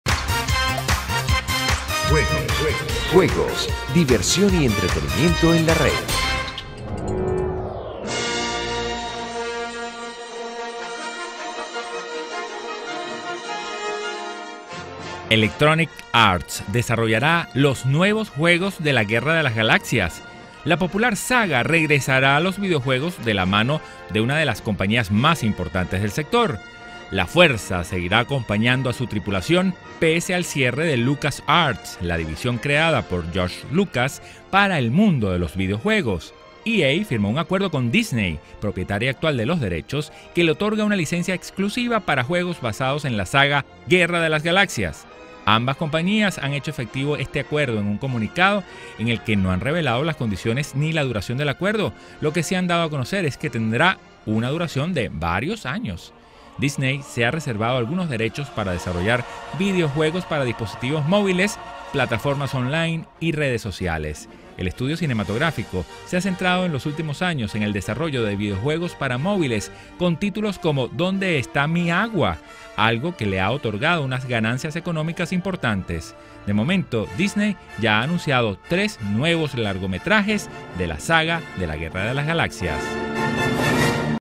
EA desarrollará los nuevos juegos de la Guerra de las Galaxias
Electronic Arts desarrollará los nuevos juegos de la Guerra de las Galaxias.  La popular saga regresará a los videojuegos de la mano de una de las compañías más importantes del sector.  La fuerza seguirá acompañando a su tripulación pese al cierre de Lucas Arts, la división creada por George Lucas para el mundo de los videojuegos. EA firmó un acuerdo con Disney - propietaria actual de los derechos - que le otorga una licencia exclusiva para juegos basados en la saga «Guerra de las Galaxias».